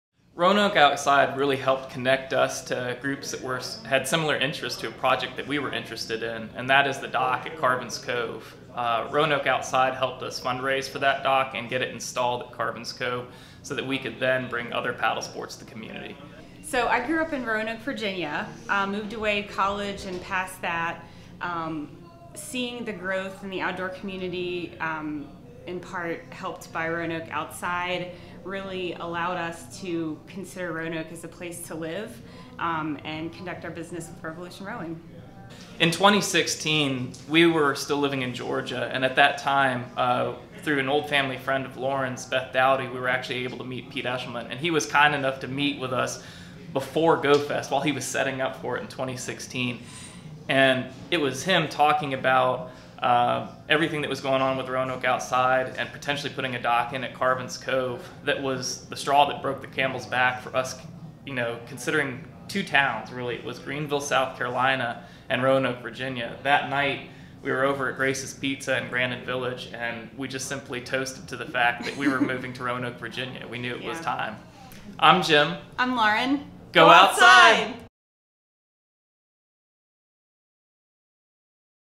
Jim and Lauren Pickens
Jim and Lauren Pickens of Revolution Rowing brought their business to the Roanoke Region, and credit Roanoke Outside with helping them make that decision.